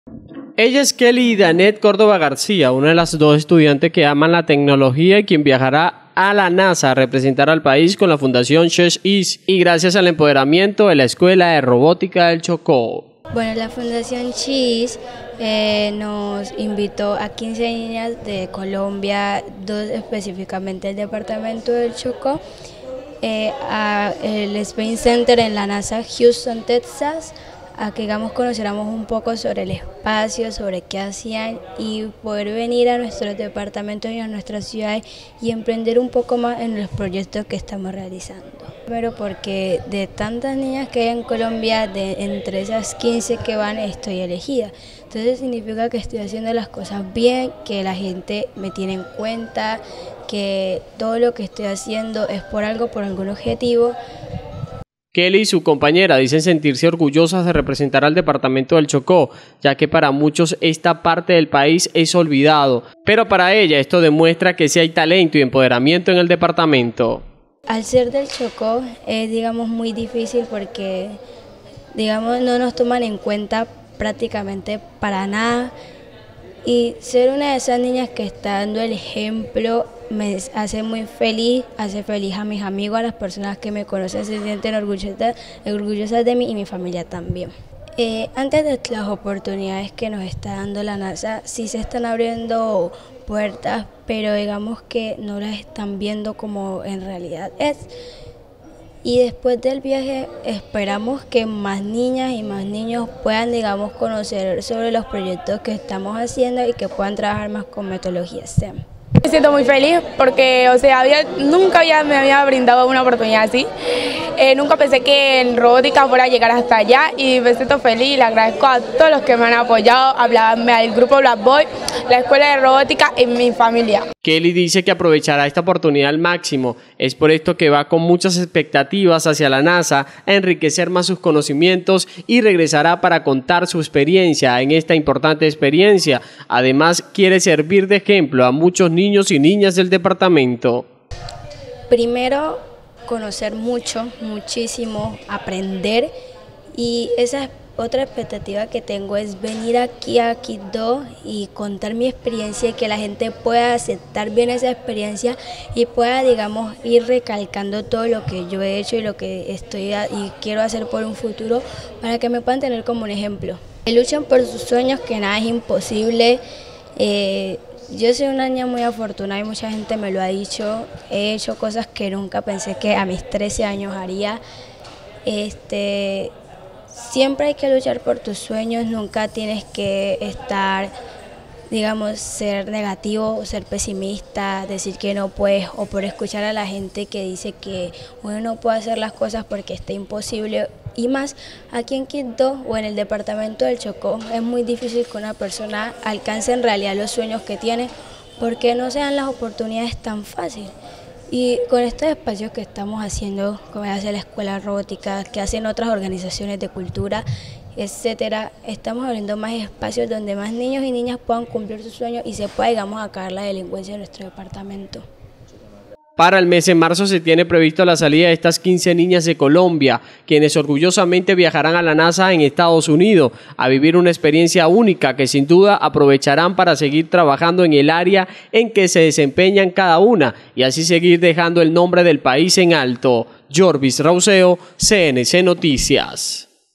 2 niñas chocoanas visitaran la nasa
Dos niñas del departamento iran a visitar a la nasa en Houston Texas junto a otras 13 de diferentes departamentos a representar al país.